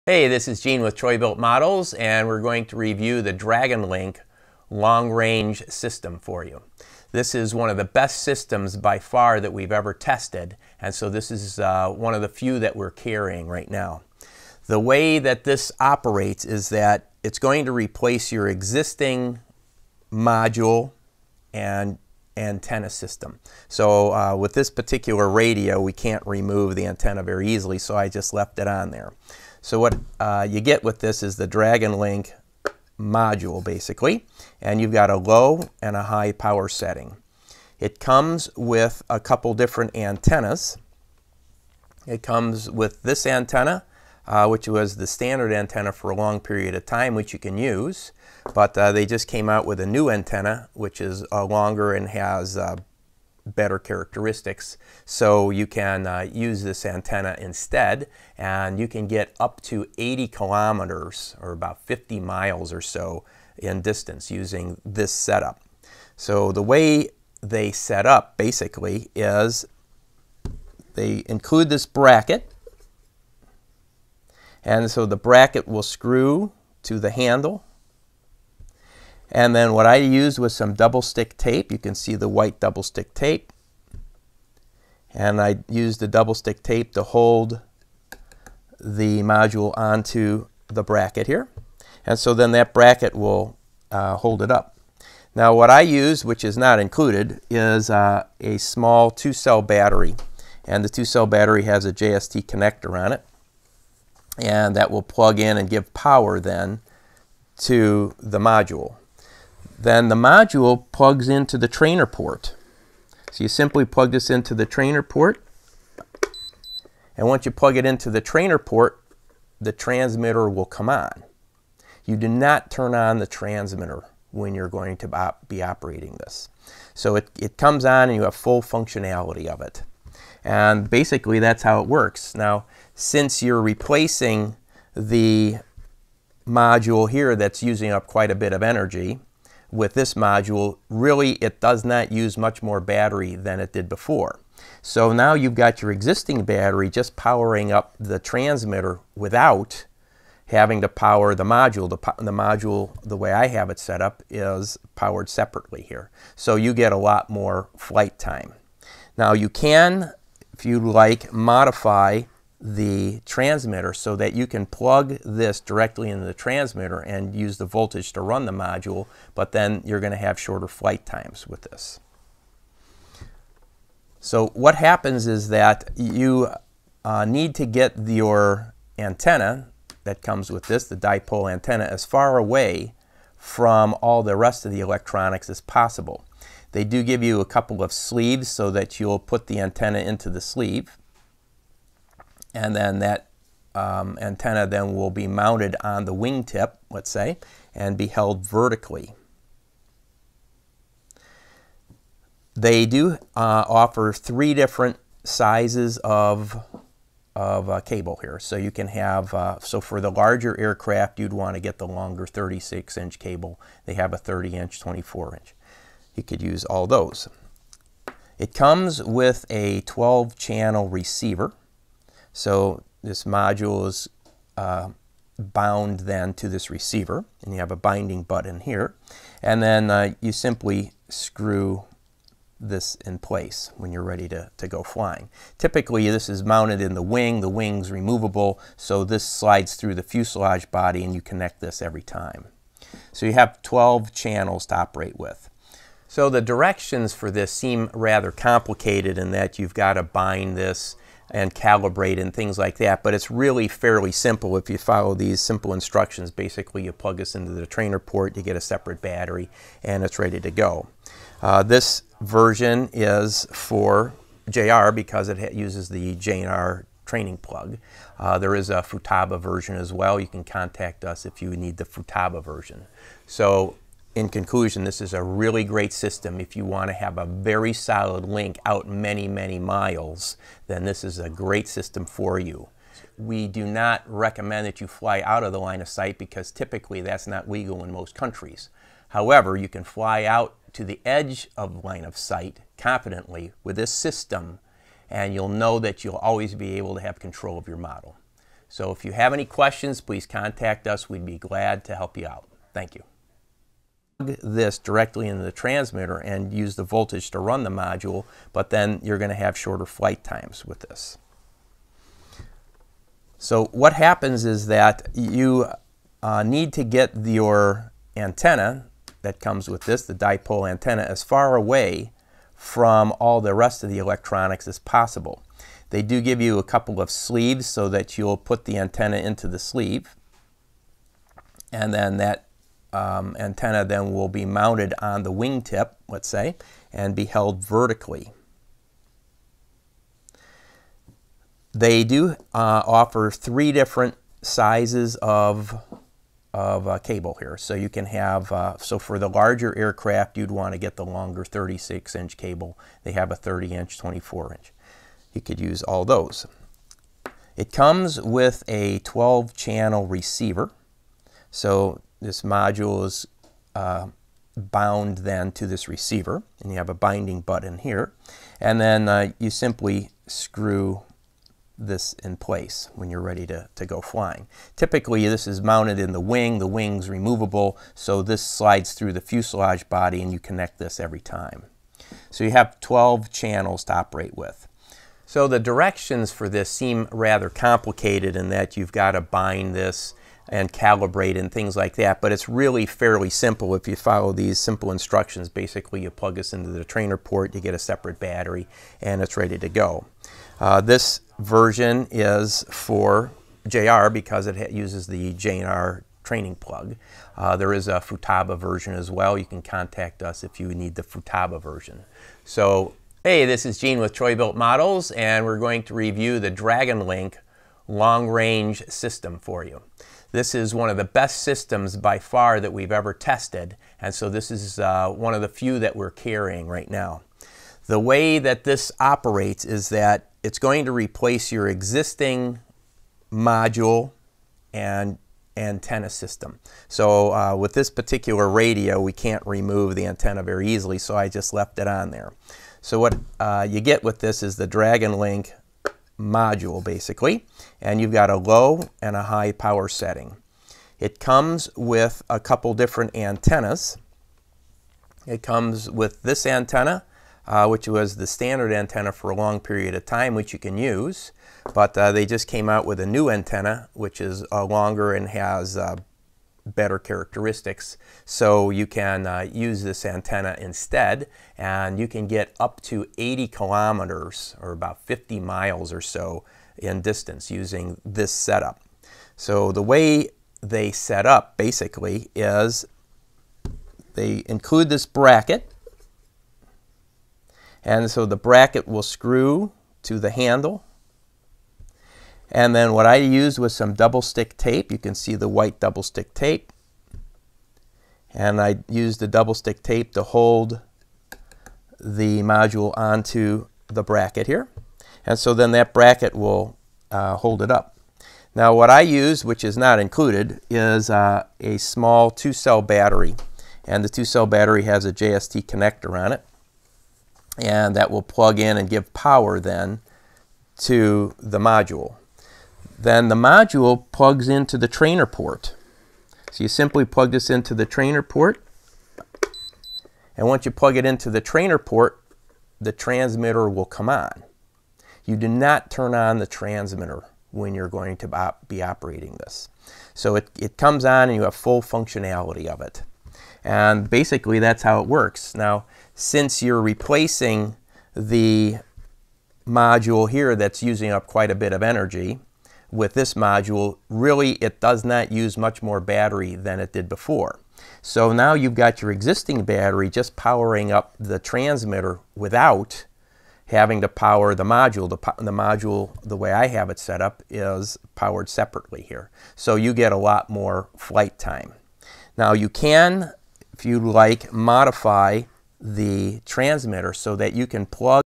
DragonLink Long Range V2 Transmitter and Receiver System for JR Radios including RX Antenn
Gene showing how you would hook up the DragonLink Long Range V2 Transmitter and Receiver System for JR Radios including RX Antenna to your JR radio.

The SWEET audio track for this vid is the sole property of Jack Conte (youtube ID jackcontemusic). The original video he composed can be found here and is .